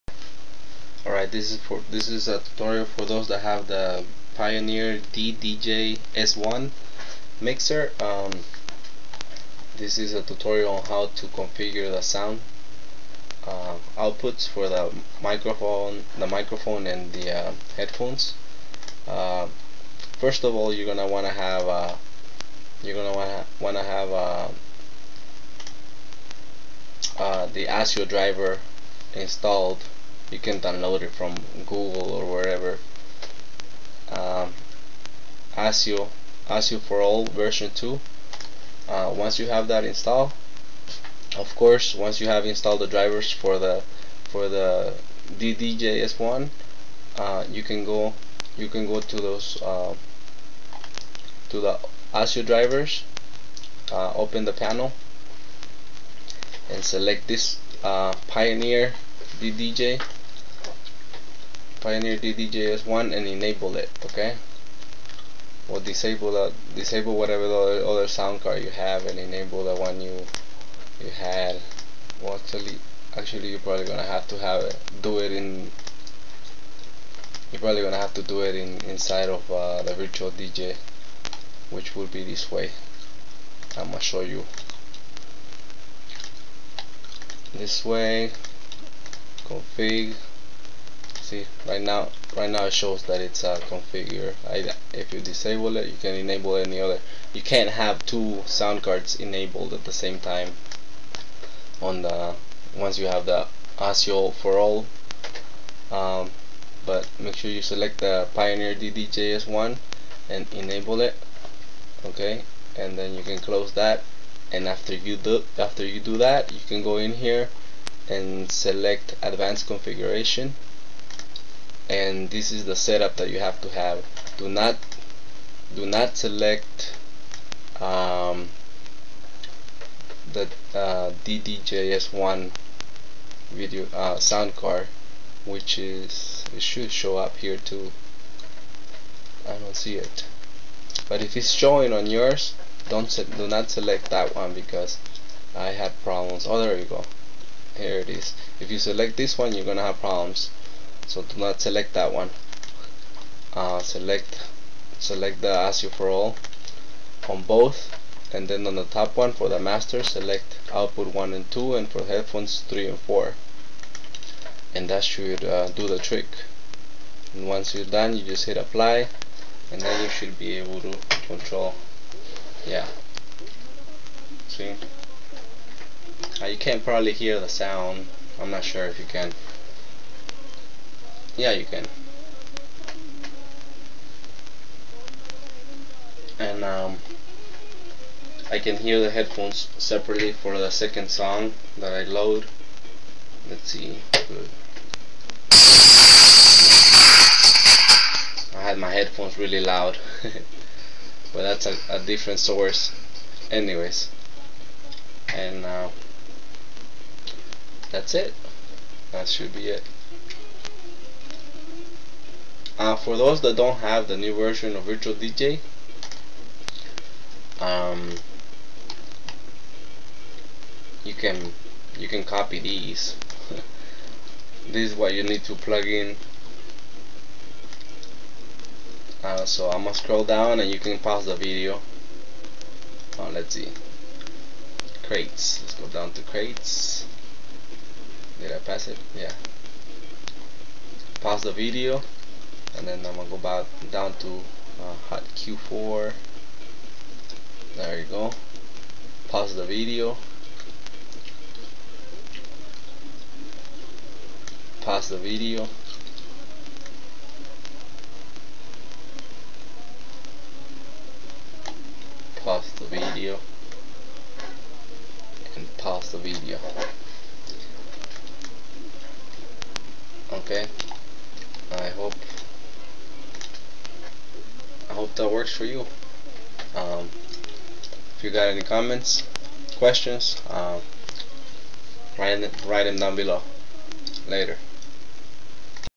Virtual DJ Pioneer ddj-s1
How to configure the headphones on Virtual DJ with the Pioneer DDJ-S1. I'm not sure if the mapping will work on older versions of vdj but i'm using vdj v7.0.5.